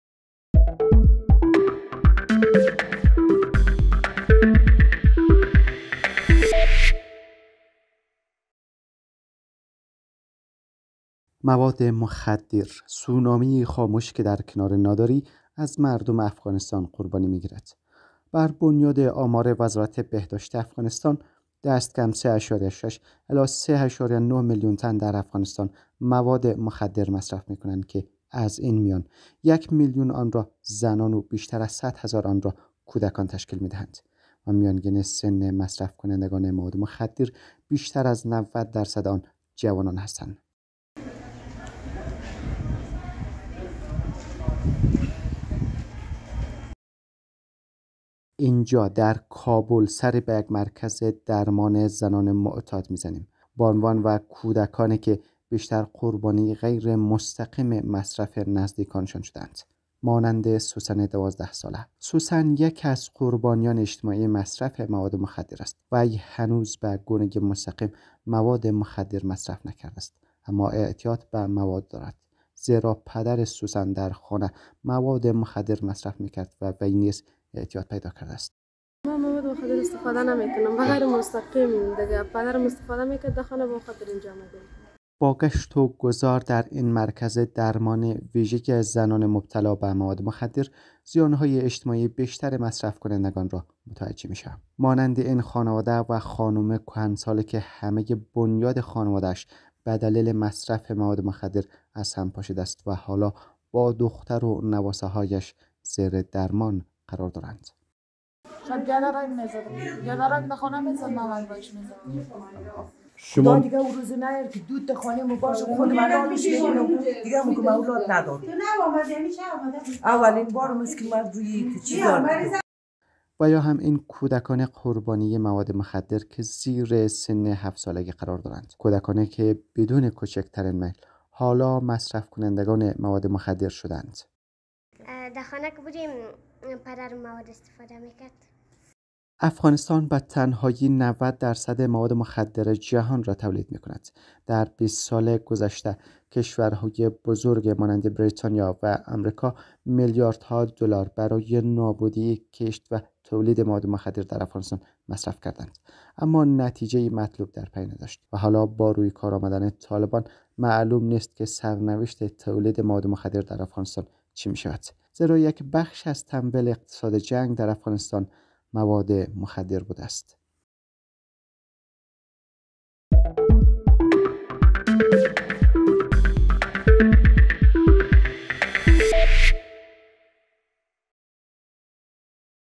گزارش از افغانستان: معضل اعتیاد در قطب تولید تریاک جهان
بهنود - سوء مصرف مواد مخدر سونامی خاموشی است که در کنار فقر، از مردم افغانستان قربانی می‌گیرد. بربنیاد آمار وزارت بهداشت أفغانستان دستکم ۳,۶ الی ۳,۹ میلیون تن درافغانستان مواد مخدر مصرف می کنند که از نظر میانگین سنی، بیش از ۹۰ درصد آنها جوانان هستند.
در میان کسانی که به سوء مصرف مواد مخدر در أفغانستان می‌‌پردازند، یک میلیون تن زن هستند و بیشتر از صد هزار تن نیز کودک.
سهم تریاک تولیدشده در افغانستان در ۲۰۲۰ حدود ۸۵ درصد تریاک تولیدشده در کل جهان بود. دربیست سال گذشته کشورهای بزرگ مانند بریتانیا وامریکا میلیارد‌ها دلار برای نابودی کشت مواد مخدر درافغانستان مصرف کردند اما نتیجه‌ی مطلوب درپی نداشت. وحالا با روی کار آمدن طالبان معلوم نیست که سرنوشت تولید مواد مخدر درافغانستان چه می‌شود؛ چرا که بخش مهمی از تمویل اقتصاد جنگ درافغانستان مواد مخدر بوده است.